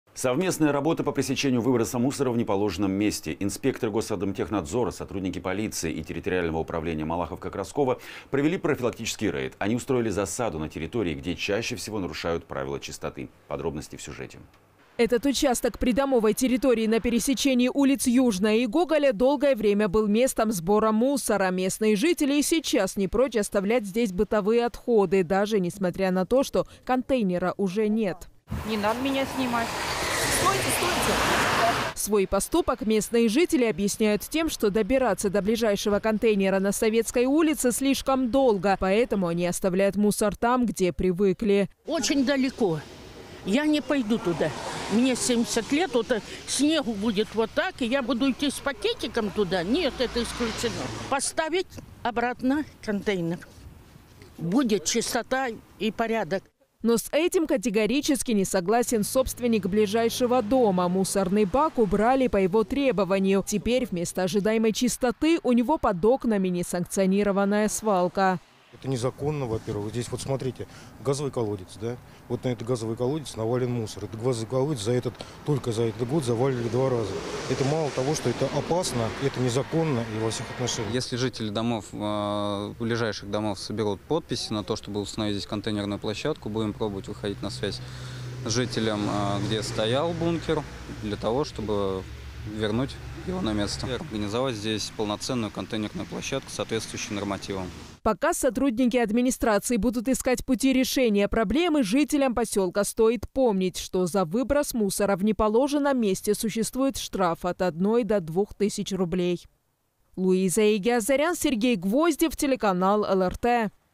Контейнер убрали, а мусор остался
Совместная работа по пресечению выброса мусора в неположенном месте. Инспекторы Госадмтехнадзора, сотрудники полиции и территориального управления Малаховка-Красково провели профилактический рейд. Они устроили «засаду» на территории, где чаще всего нарушают  правила чистоты. Подробности –в сюжете.

Этот участок придомовой территории на пересечении улиц Южная и Гоголя долгое время был местом сбора мусора. Местные жители и сейчас не прочь отставлять здесь бытовые отходы, даже несмотря на то, что контейнера уже нет.

Свой поступок местные жители объясняют тем, что добираться до ближайшего контейнера на Советской улице слишком долго. Поэтому они оставляют мусор там, где привыкли.

Нина Будко, жительница г.о. Люберцы:
«Очень далеко. Я не пойду туда. Мне 70 лет, снегу будет вот так и я буду идти с пакетиком туда? Не, это исключено. Поставить обратно контейнер, будет чистота и порядок».

Но с этим категорически не согласен собственник ближайшего дома. Мусорный бак убрали по его требованию. Теперь вместо ожидаемой чистоты у него под окнами несанкционированная свалка.

Андрей Лужников, житель г.о. Люберцы:
«Это незаконно, здесь газовый колодец, вот на этот газовый колодец навален мусор, только 0за этот год заваливали 2 раза, мало того, что это опасно, это незаконно во всех отношениях».

Никита Коваленко, ведущий специалист отдела ЖКХ территориального управления Малаховка-Красково:
«Если жители ближайших домов соберут подписи на то, чтобы установить здесь контейнерную площадку, будем пробовать выходить на связь с жителем, где стоял бункер, для того чтобы вернуть его на место, организовать здесь полноценную контейнерную площадку, соответствующую нормативам».

Пока сотрудники администрации будут искать пути решения проблемы, жителям поселка стоит помнить, что за выброс мусора в неположенном месте существует штраф от 1 до 2 тысяч рублей.